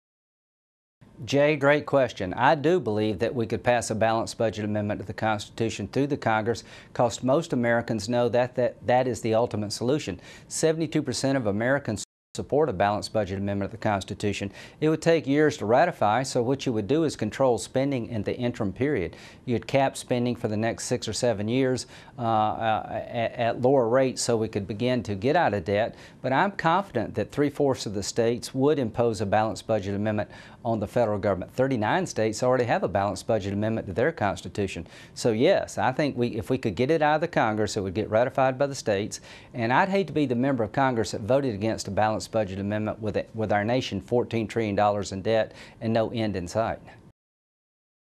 Is a Balanced Budget Amendment Achievable?
U.S. Senator Lindsey Graham responds to a Facebook question from Jay who asks, "Do you think a balanced budget amendment is achievable in the near future?"